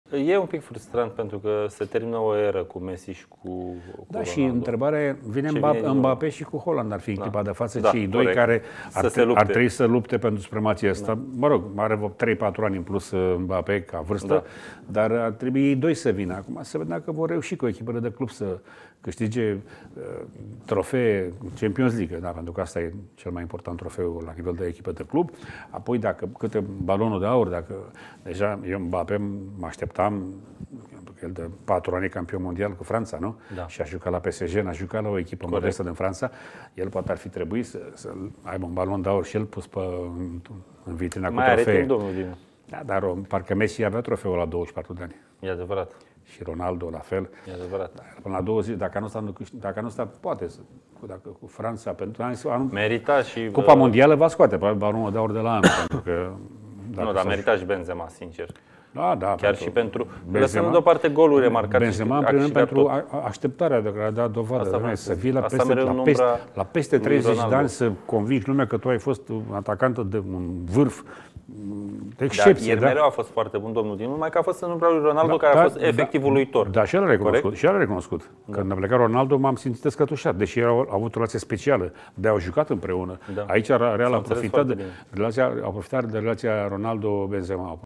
"Messi și Ronaldo aveau Balonul de Aur la 24 de ani, Mbappe nu!"
Dinu Gheorghe îl compară pe Mbappe cu marile staruri ale fotbaului.